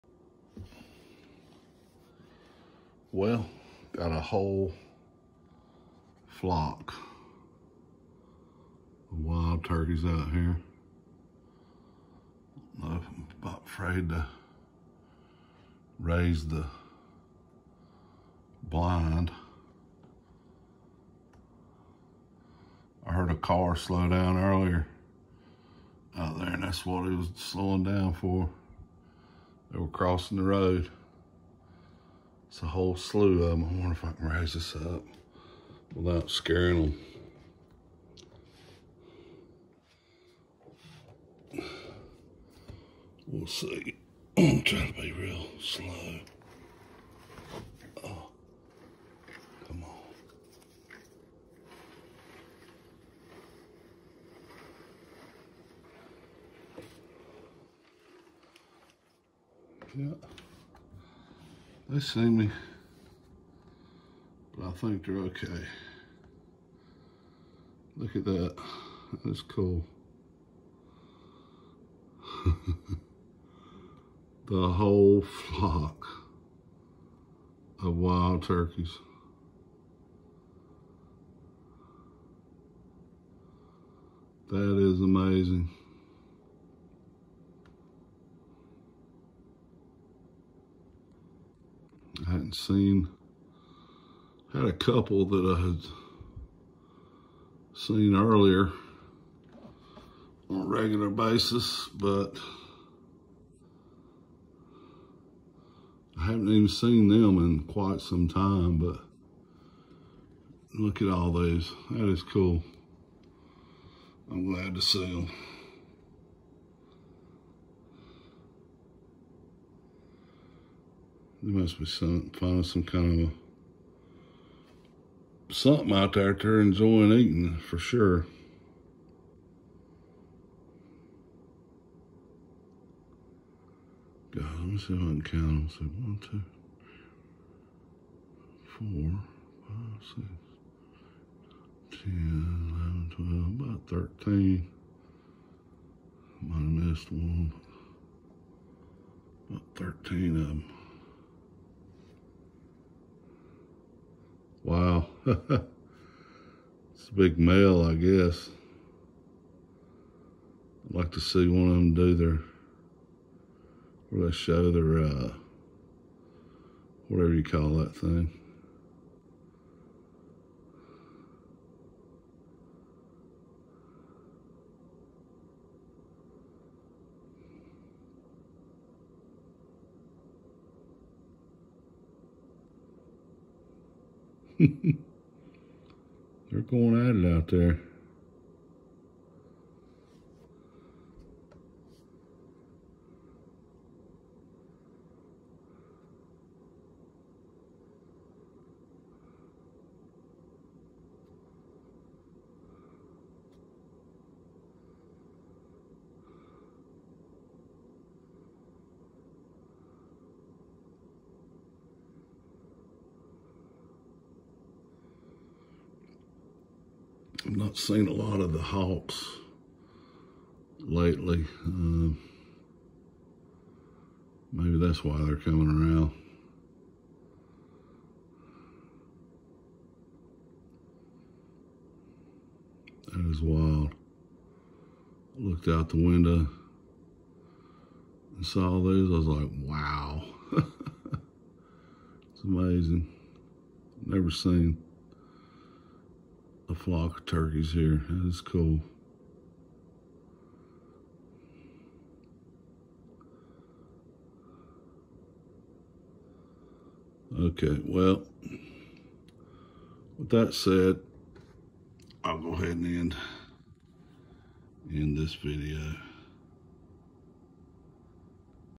Wild Turkeys 8-20-23
Captured a video of a Wild Turkey flock. I have seen the flock before up the road a ways, but never in my front yard. It was good to see them.